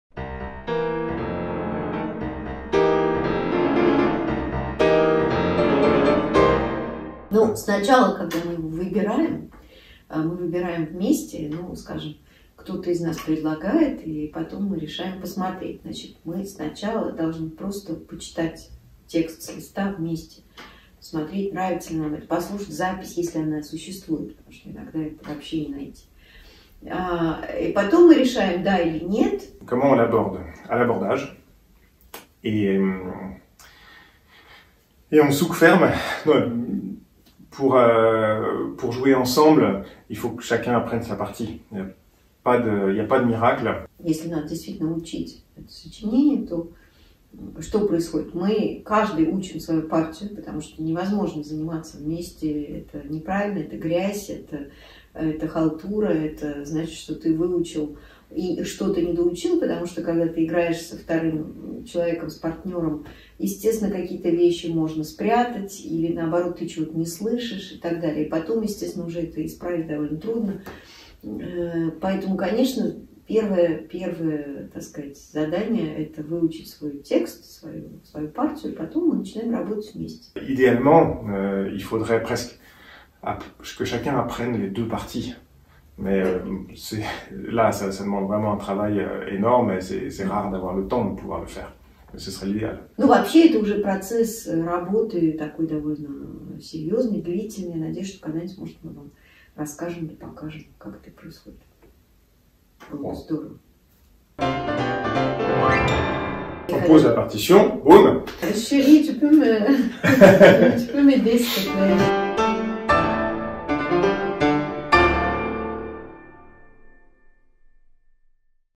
Clichés of Piano Duo - Episode #3
Episode #3 : How do you tackle a new piece ? / Comment abordez-vous une oeuvre ?

Ludmila Berlinskaya & Arthur Ancelle, pianists

We've noticed that people are always curious about piano duo performances, especially on 2 pianos, we get a lot of questions from the audience after the concerts. We also noticed that even among music lovers and professionals working in the music field, piano duo remains a bit of a mystery...
So we asked acquaintances to send us their questions : what do you want to know about piano duo ?

Arthur answers in French and Ludmila answers in French or Russian, depending on the question, or depending on her mood.

ピアノデュオ ピアノデュオ& アーサーアンセル
фортепианный дуэт Людмила Берлинская & Артур Ансель
피아노 듀오 루드밀라 베를린 스카 야 & 아서 앙셀